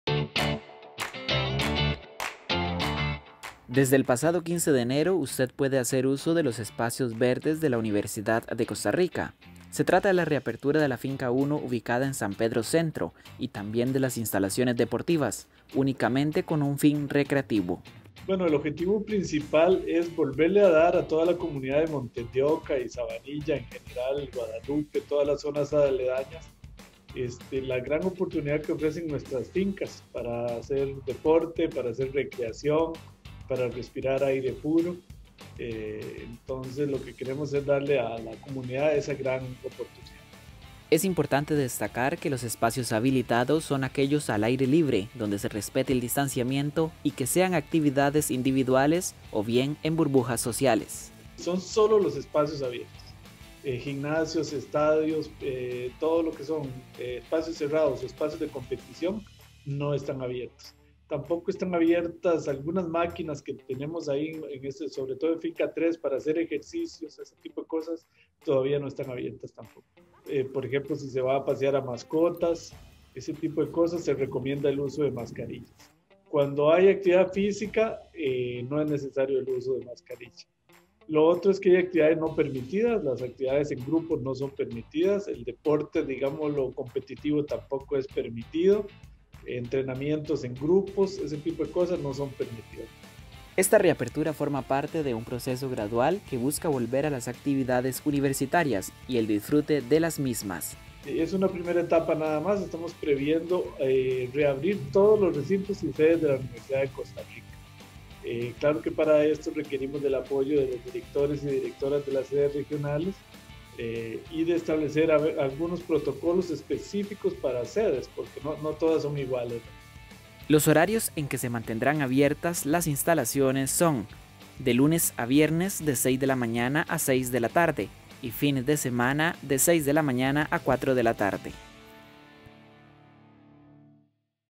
Reapertura de instalaciones UCR
La UCR pone a disposición sus instalaciones para el disfrute de actividades de recreación. Las personas que deseen hacer uso de los espacios, deben cumplir con las medidas de salud establecidas por el Ministerio de salud.